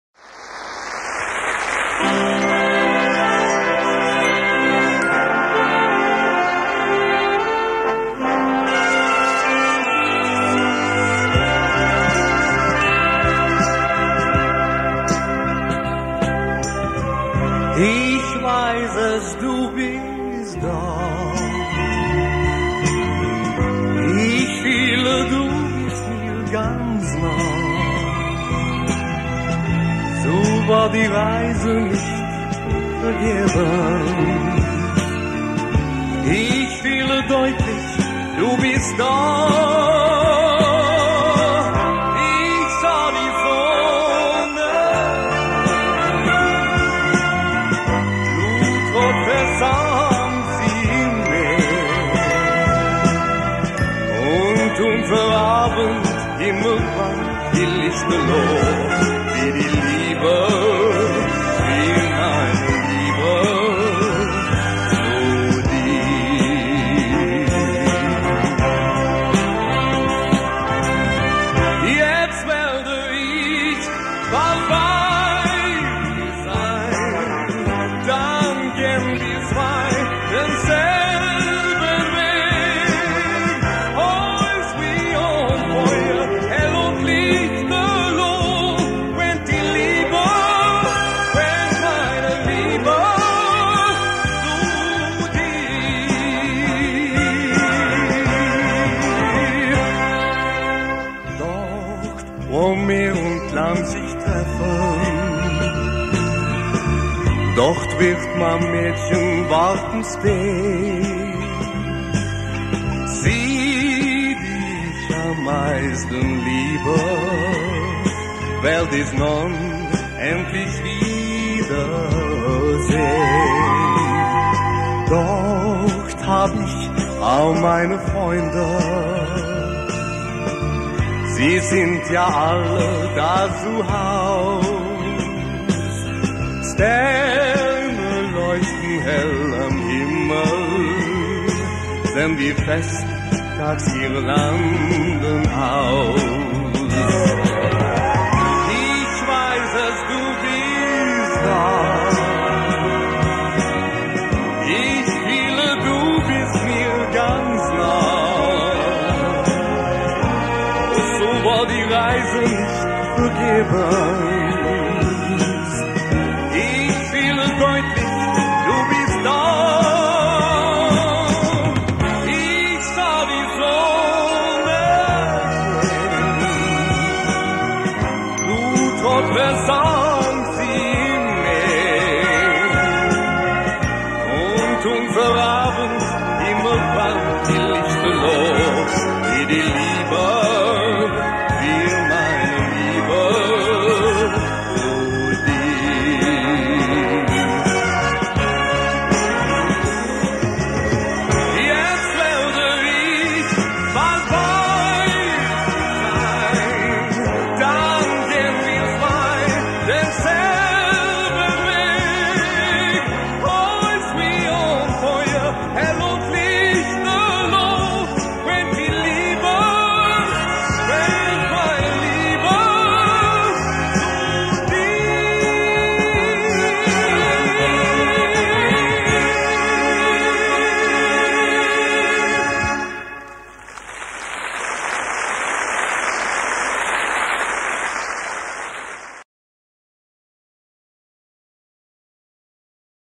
SVENN ERIK FJELDBERG
Livekonsertopptak  fra et tysk TV show.